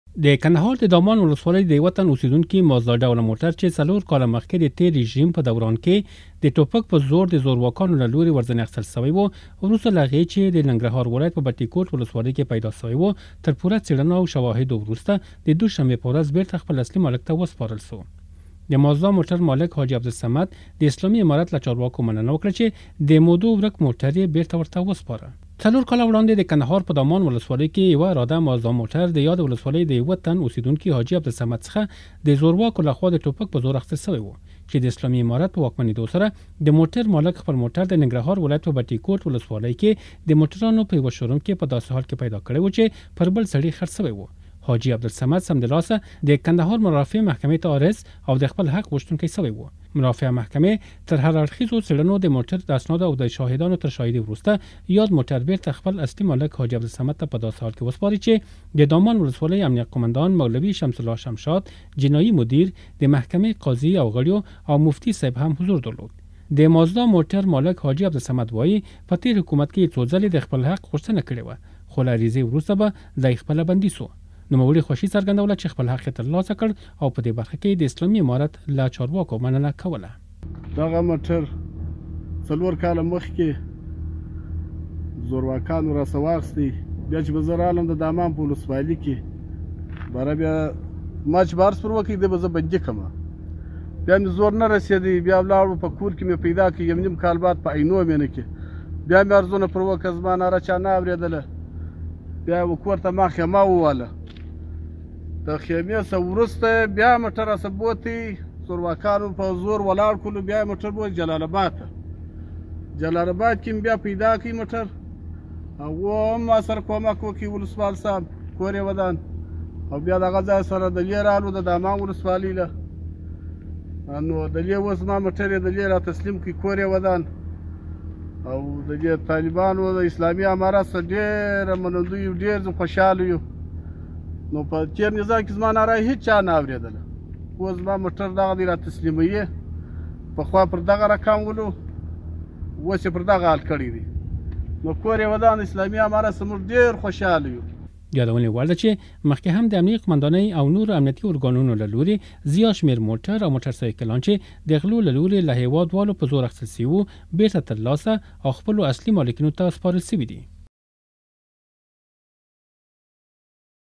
دکندهار د دامان ولسوالۍ د یوه تن اوسيدونکي مازدا ډوله موټر له پیدا کیدو وروسته خپل مالک ته وسپارل سو
د کندهار د دامان ولسوالۍ د یوه تن اوسيدونکي مازدا ډوله موټر له پیدا کیدو وروسته خپل مالک ته وسپارل سو